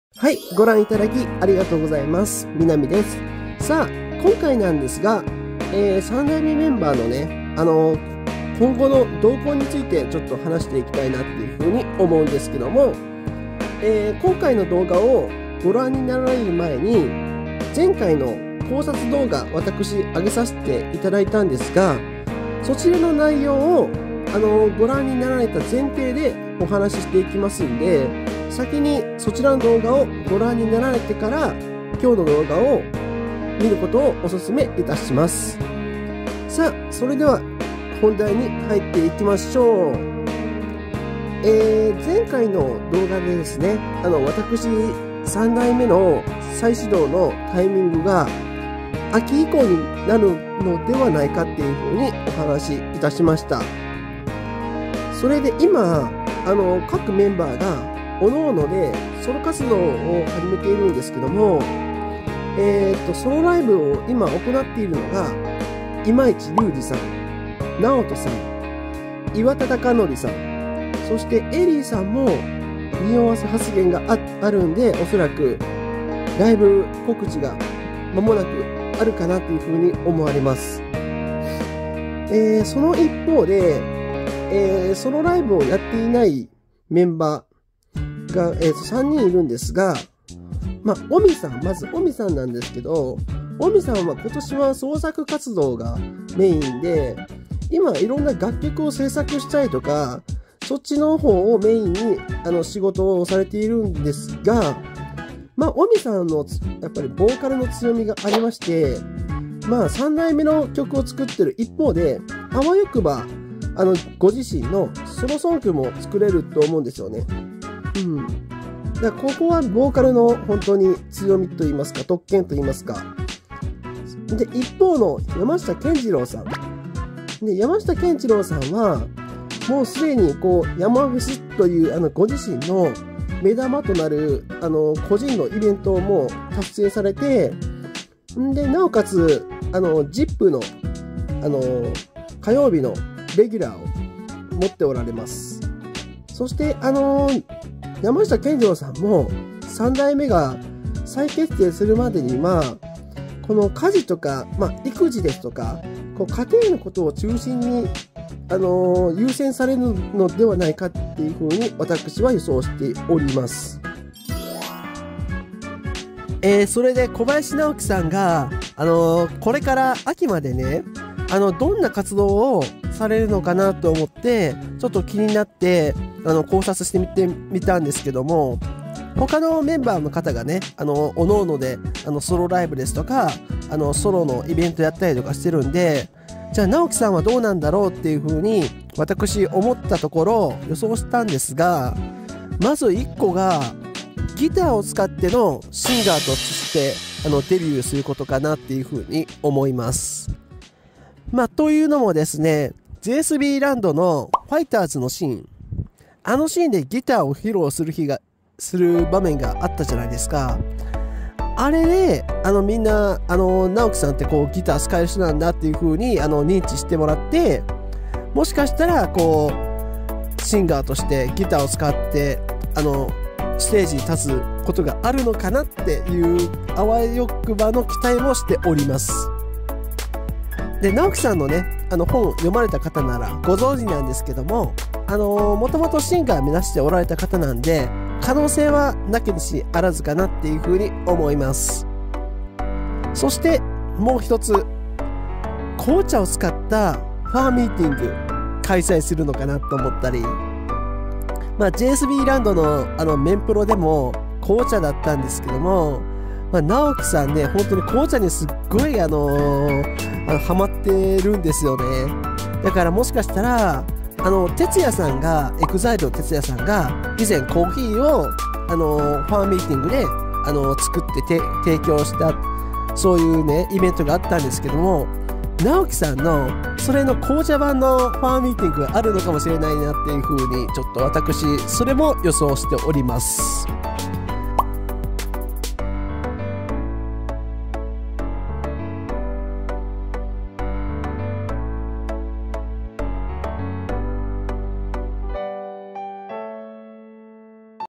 【予想】三代目JSBが再始動するまで、ソロイベントが発表されていないメンバーの今後はどうなるのか？
※パフォーマー組はソロライブではなく、ソロイベントじゃないか？という意見があるかもですがタイトル省略の為、ライブで統一しております。